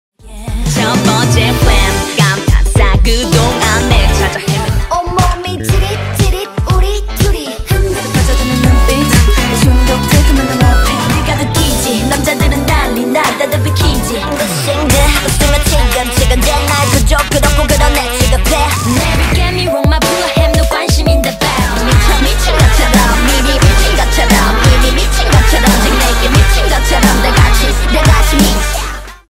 2015 Female Rappers(Sneak Peek)
Here's a sneak peek at the new video I'm making.

It's of the rappers in female groups. The videos are from the first half of the year. It'll have lots of rappers from different groups, so be on the look out for it!

I will also be doing this for the male groups as well. If these do well, I may do videos for the second half as well.